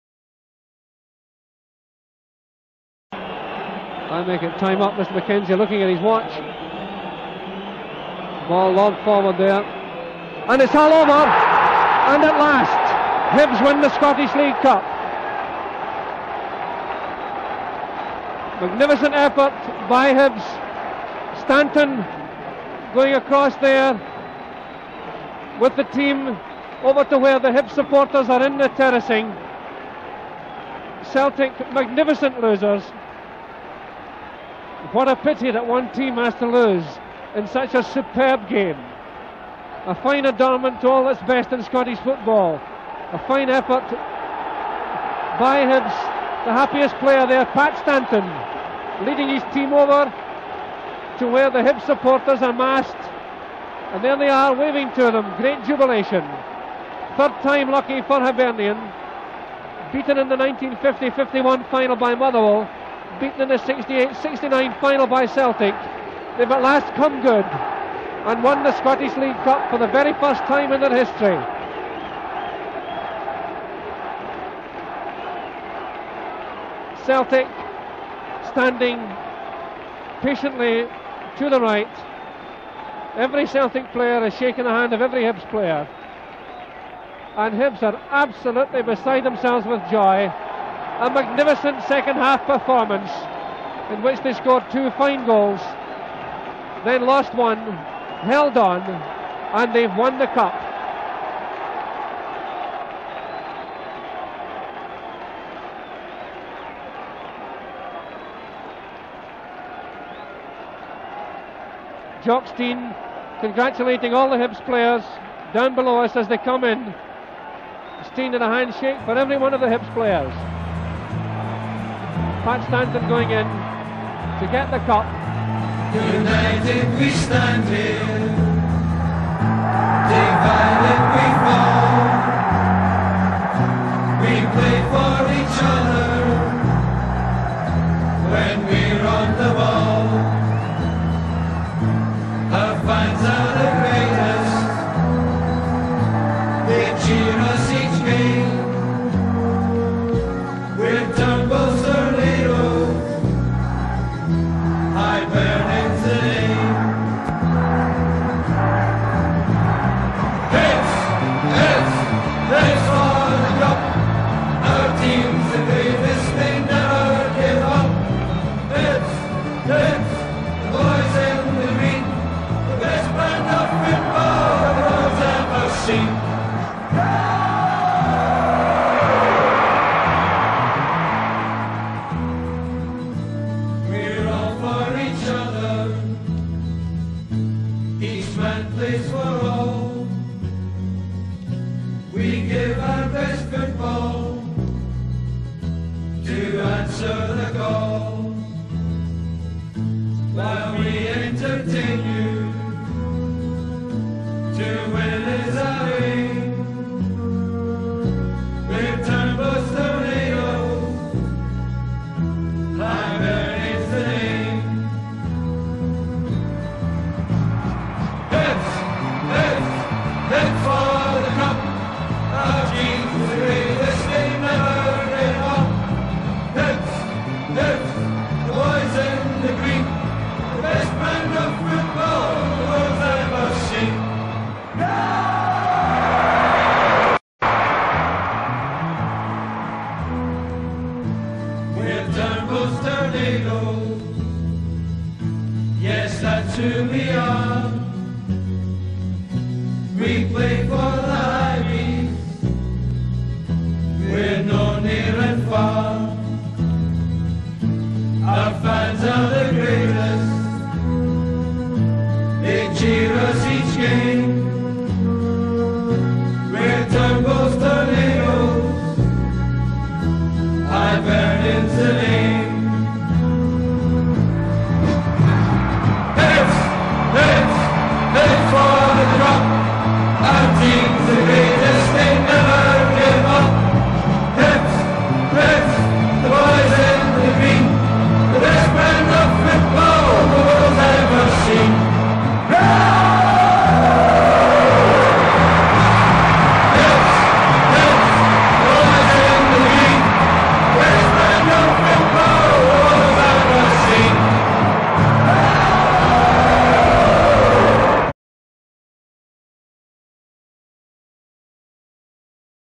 New: 42 Years On - Tornadoes' Triumph. Hibs TV Exclusive
An early Christmas present for Hibs TV viewers – footage of the 1972 League Cup triumph; 42 years on. Hibs TV pays tribute to Turnbull’s Tornadoes – enjoy!